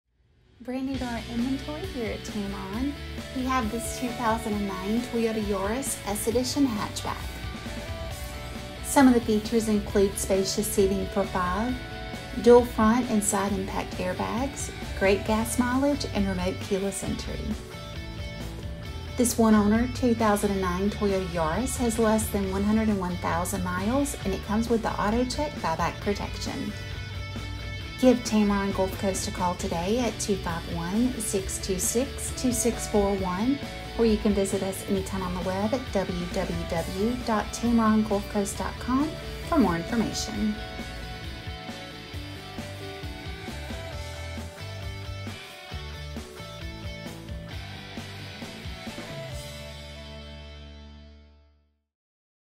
2009 Toyota Yaris S Hatchback for Sale!!!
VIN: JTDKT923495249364
Stock: C49364D

Brand new to our inventory here at Tameron Gulf Coast, we have this 2009 Toyota Yaris S Hatchback.
Some of the features include, spacious seating for five, dual front and side impact airbags, great gas mileage and remote keyless entry.
This one owner 2009 Toyota Yaris has less than 101,000 miles and comes with the AutoCheck buy back protection guarantee.